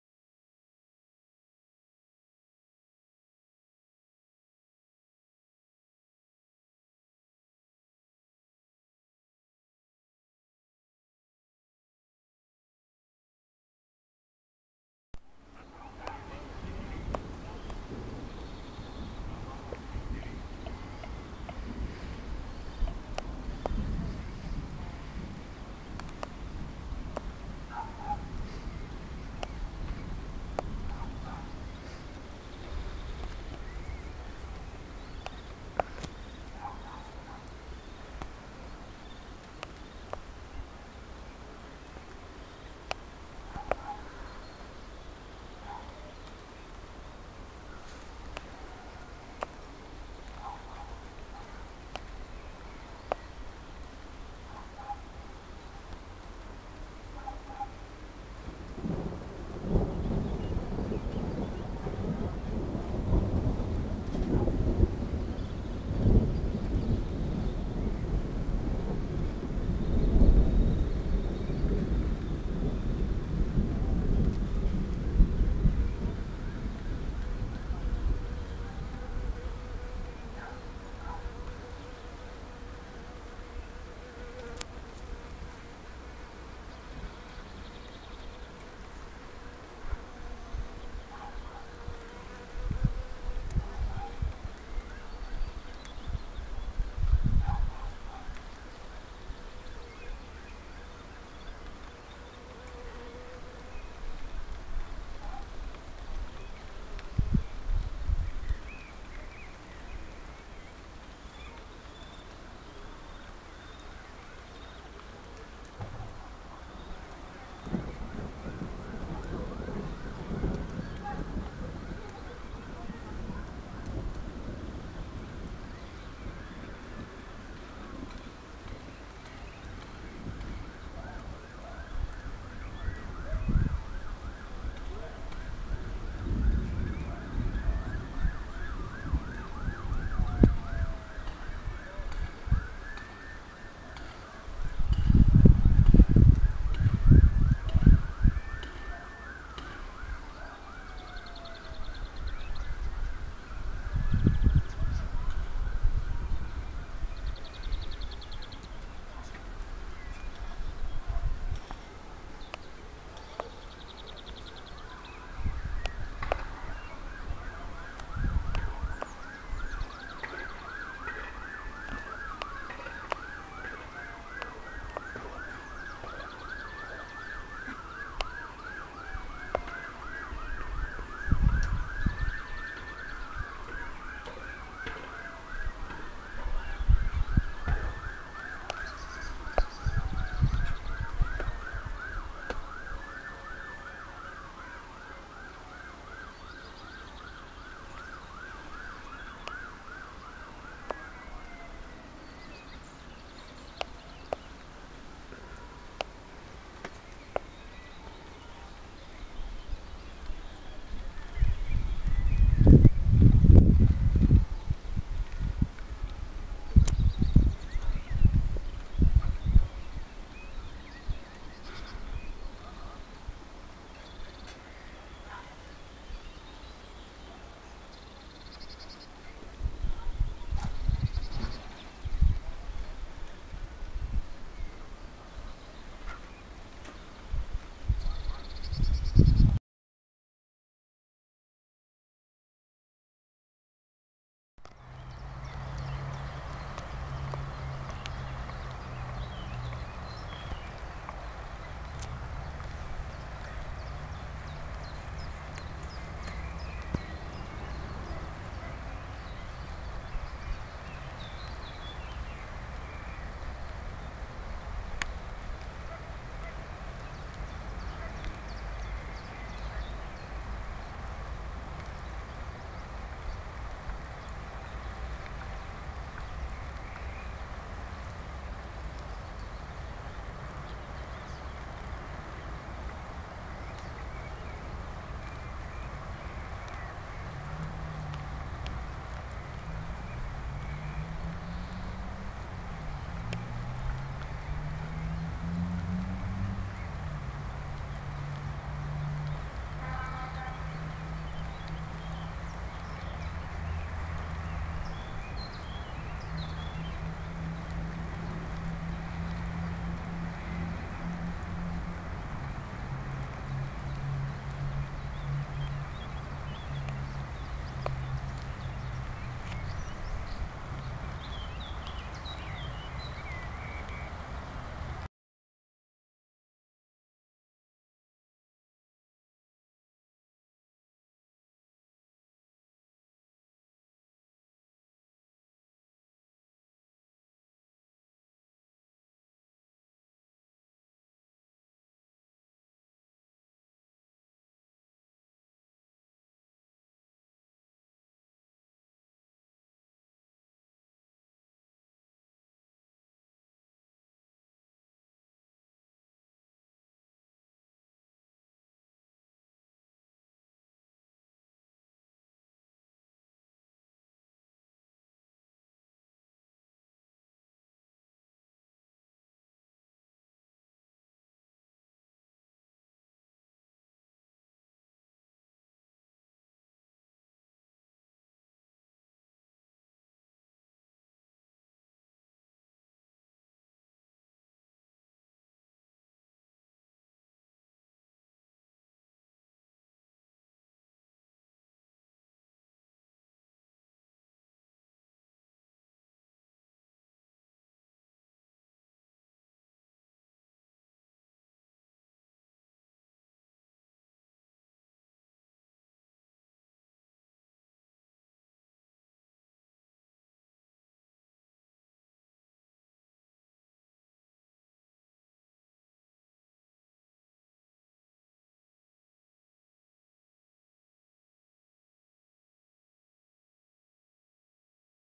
Stormchasing 26.4.2014
Všechny informace a místo pozorování jsou uvedené ve videu. Ty dva zmíněné čárové CG blesky jsou nejisté, protože v době pozorování už většina bleskové aktivity vyprchala.